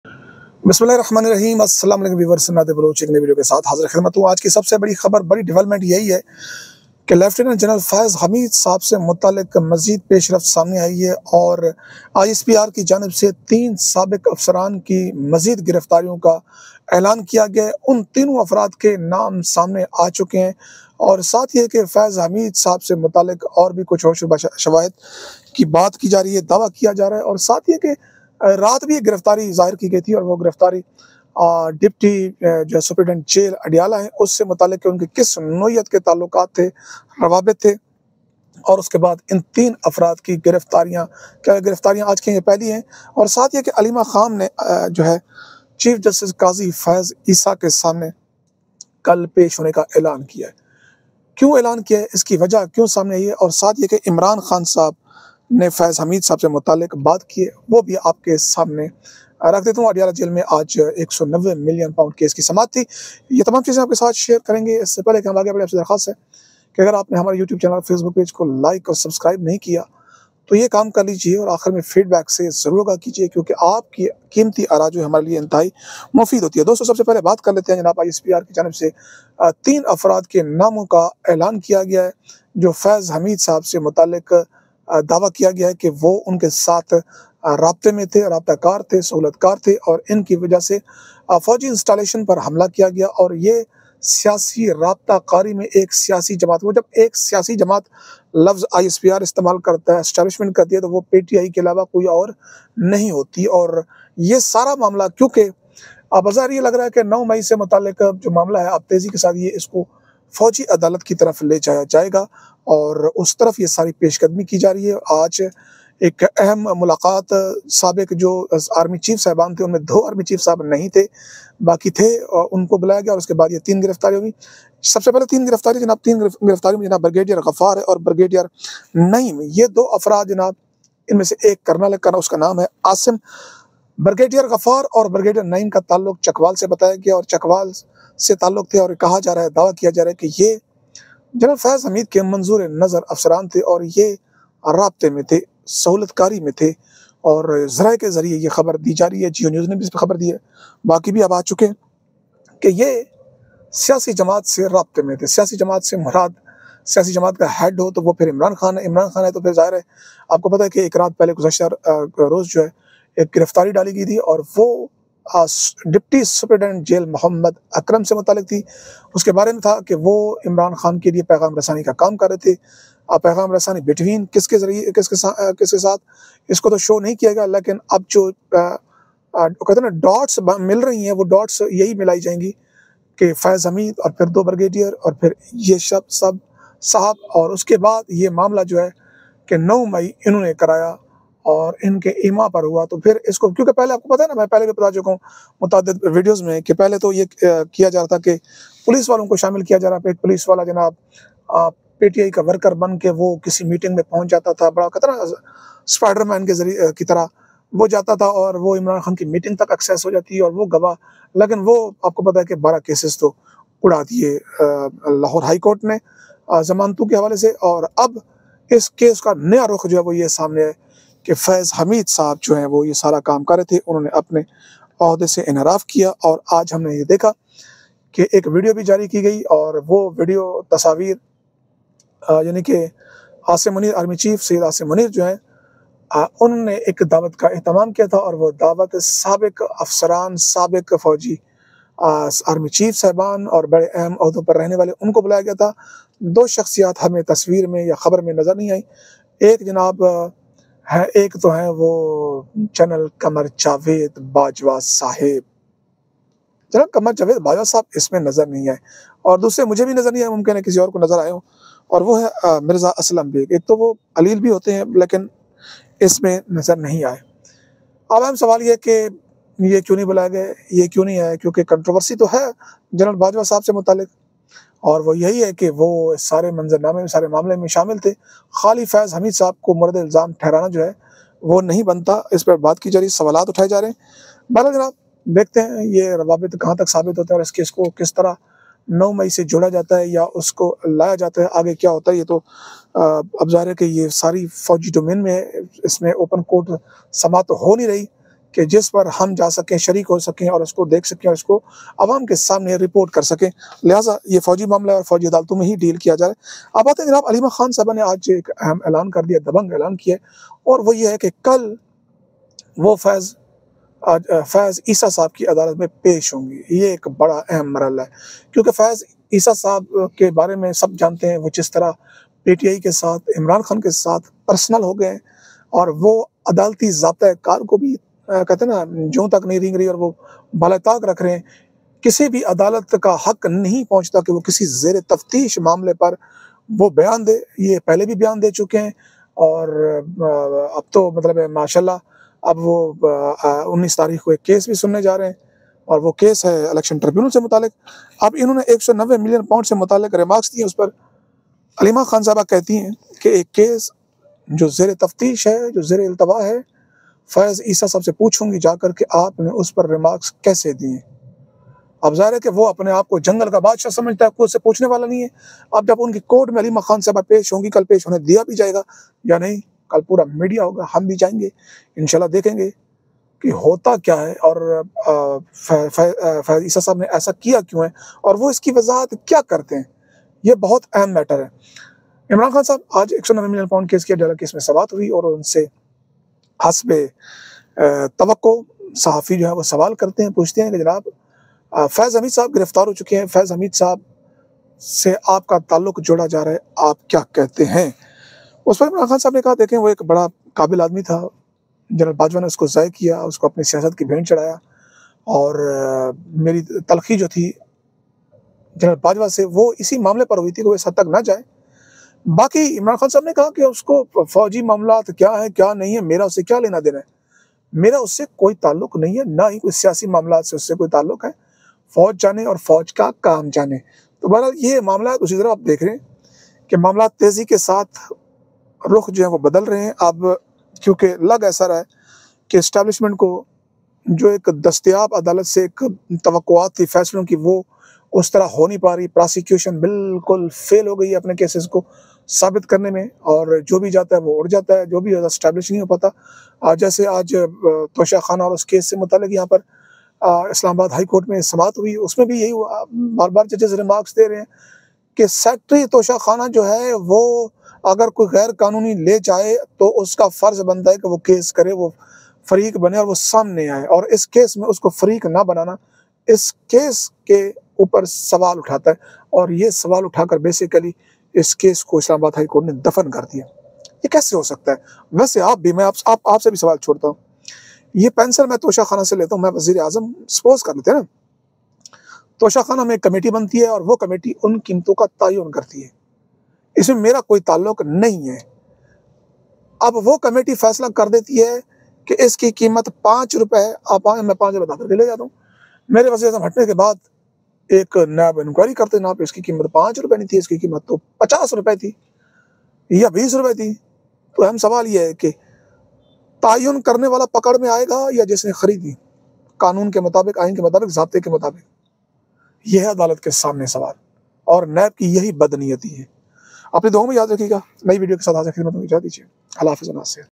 Three Ex-Army Officers Arrested: Latest Updates on Faiz Hameed Case l Imran Khan | Nadir Baloch
In this episode, Nadir Baloch provides breaking news on the arrest of three former army officers in connection with the Faiz Hameed case. He also discusses the latest developments surrounding Aleema Khan and Imran Khan. Watch for an in-depth analysis of these significant events that are making headlines across Pakistan.